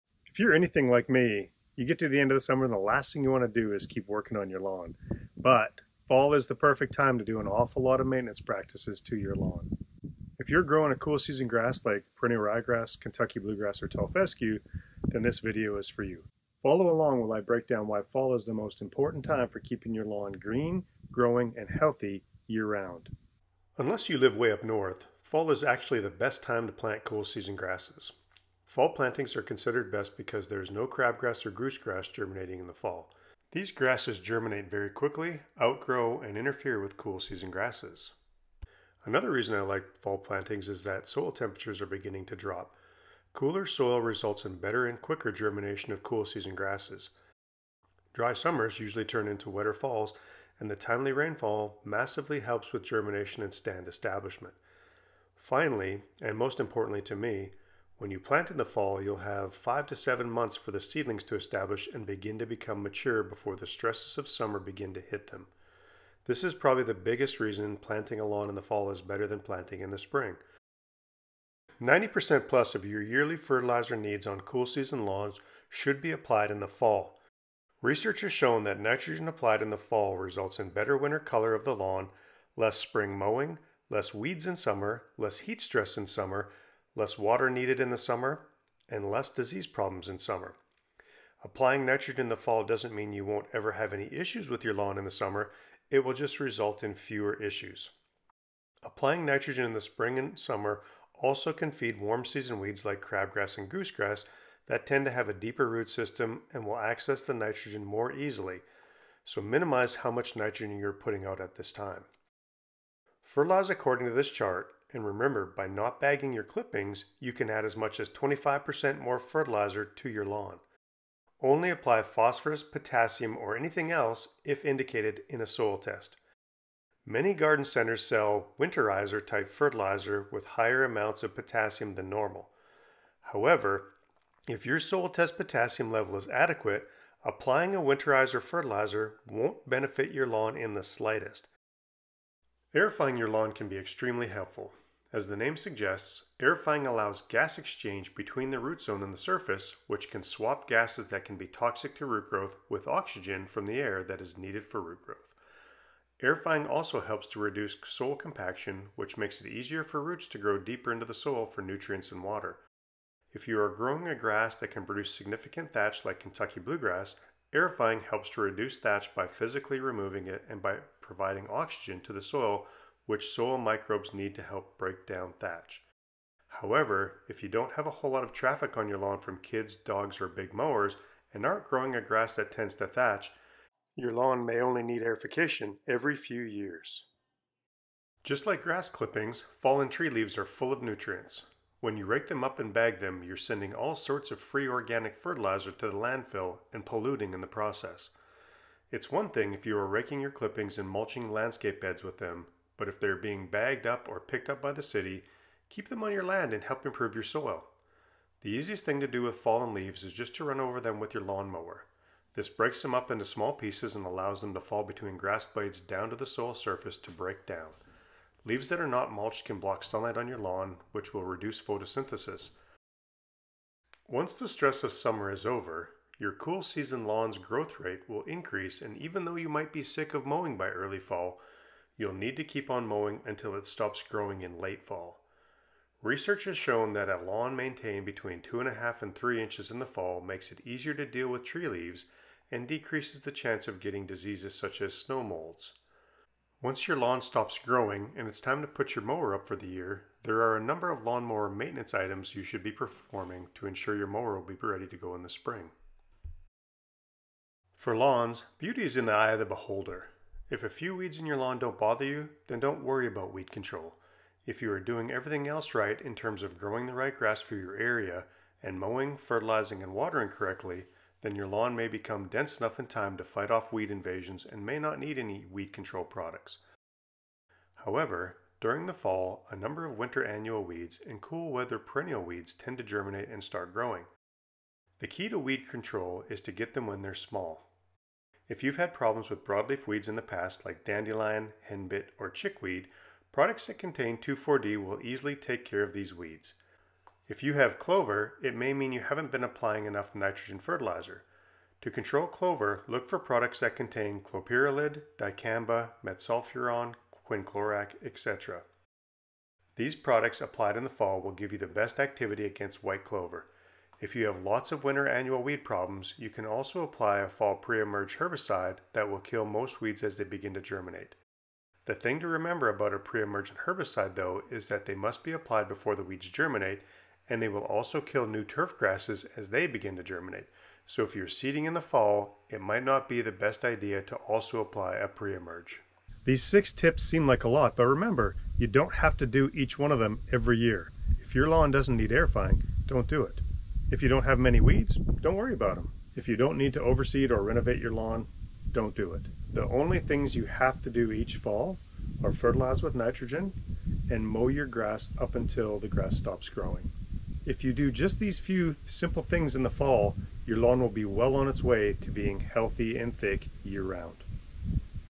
Six things to do to your lawn in the fall
This video breaks down 6 maintenance practices that your lawn may need each fall. Maintenance in the fall is critical to keeping your lawn healthy throughout the entire year. The six maintenance tips discussed in this video are: 1. Seeding/renovation 2. Fertilizing 3. Aerification 4. Removing tree leaves 5. Mowing and mower maintenance 6. Weed control

If some care is paid to these 6 topics, your lawn can be on the road to happy and healthy!

For more information on the above topics, check out these Grass Doctor videos:

Mower maintenance